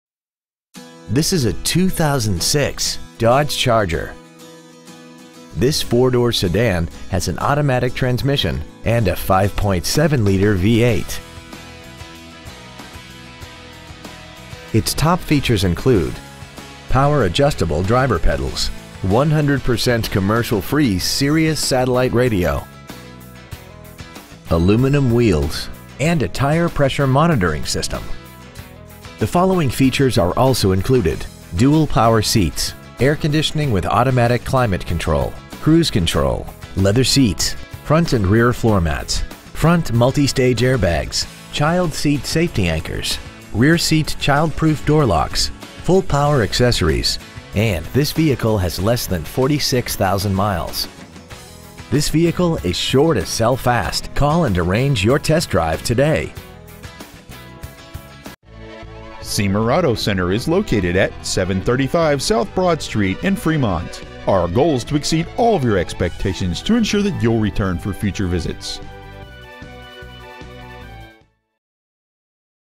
Pre-Owned 2006 Dodge Charger Fremont NE 68025
We have been honored to serve the Fremont NE area , we promise that your experience at our dealership will exceed your expectations ! Year : 2006 Make : Dodge Model : Charger Engine : 5.7L Hemi Multi-Displacement V8 Engine Trans . : Automatic Exterior : White Miles : 45,226 Interior : Gray Stock : A1920 Siemer Auto Center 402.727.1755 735 S. Broad St Fremont , NE 68025